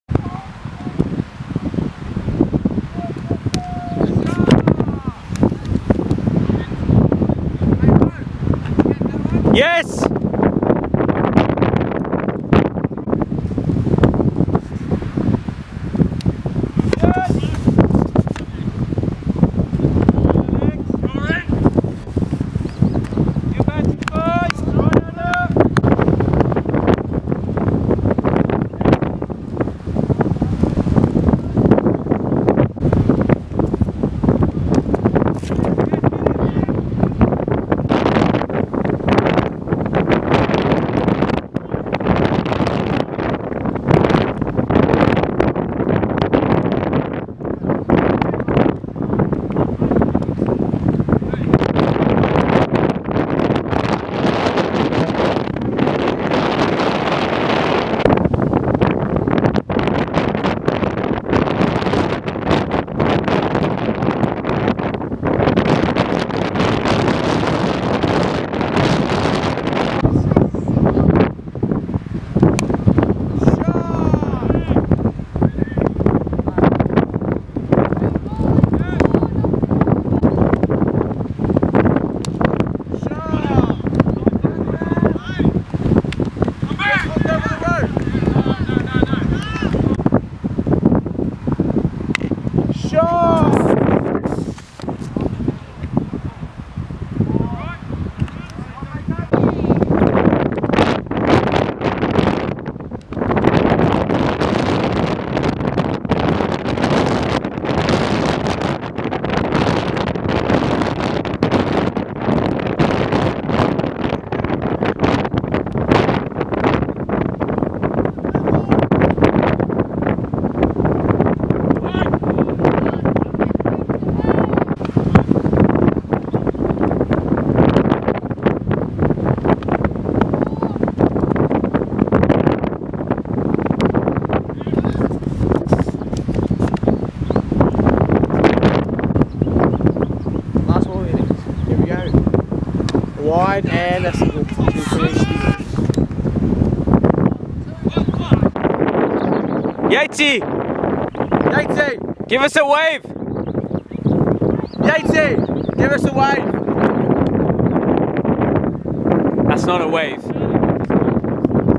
Yatesy 41 4x4s 2x6s
David hammers some big ones to get the score to 186